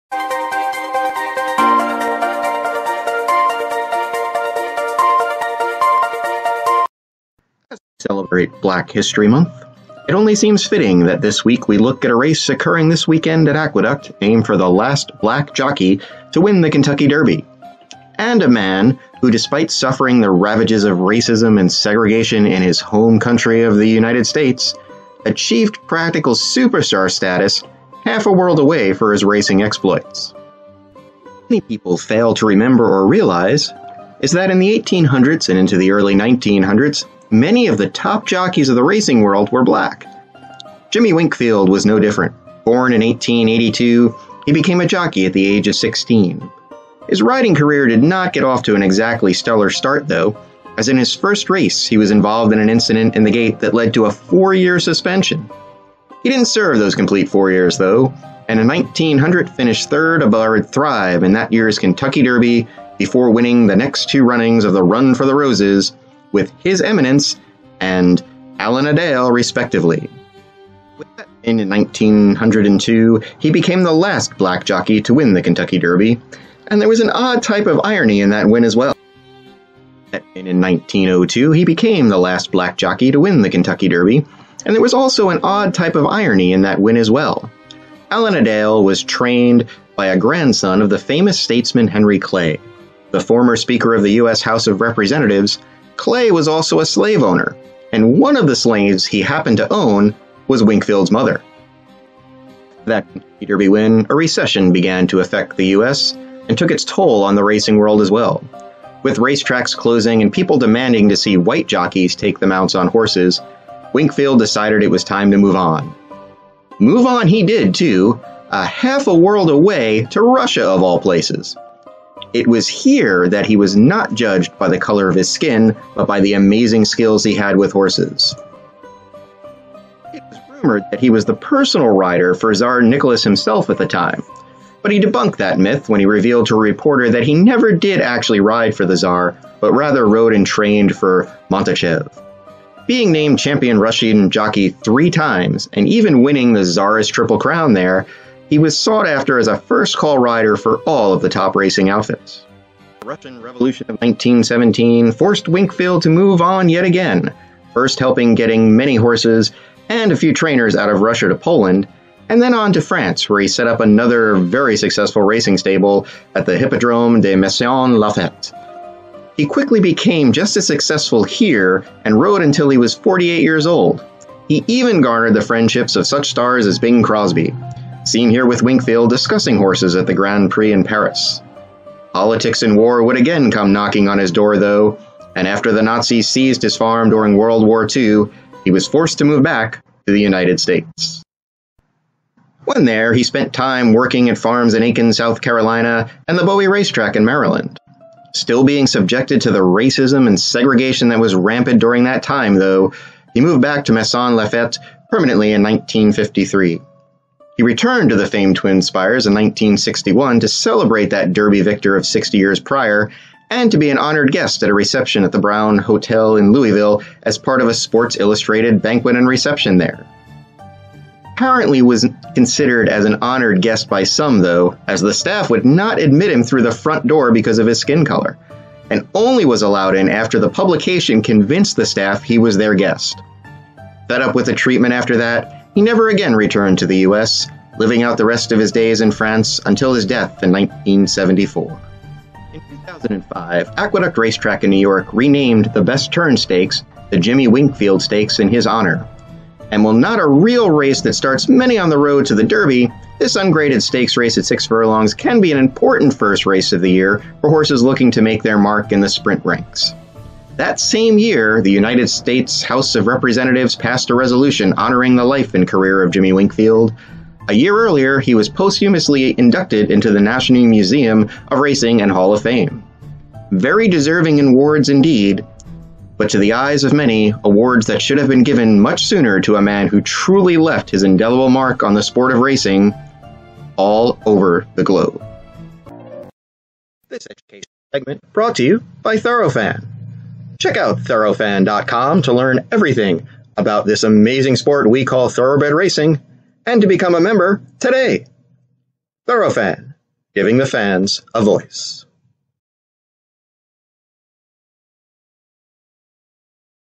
The Name Behind the Race: The Jimmy Winkfield Stakes
In this weeks Segment for Thorofan, Doc looks at a race named for a jockey that never got the respect he truly deserved while riding in the US, but got all he deserved oversees in his racing career.  Watch to see what he means.